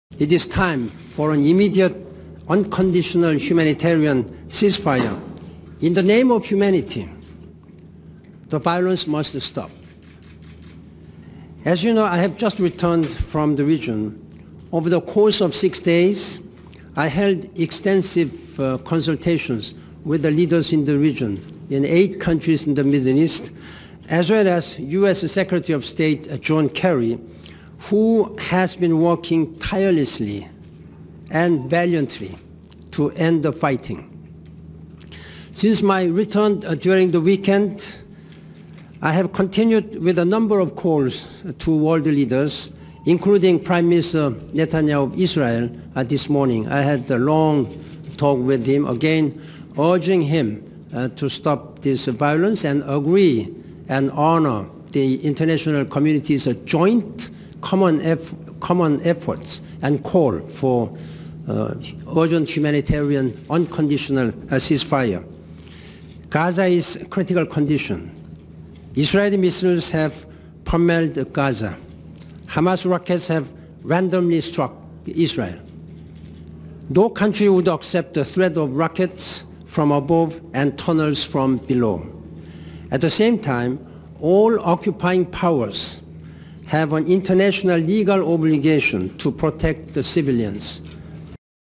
Ban Ki Moon : Stop the Violance in Gaza
Ban Ki-moon as a UN Secretary General told the violence in Gaza must stop. The critical condition in Palestinian after the two explosions one  in children’s playground and other  nearby The Gaza’s main hospital.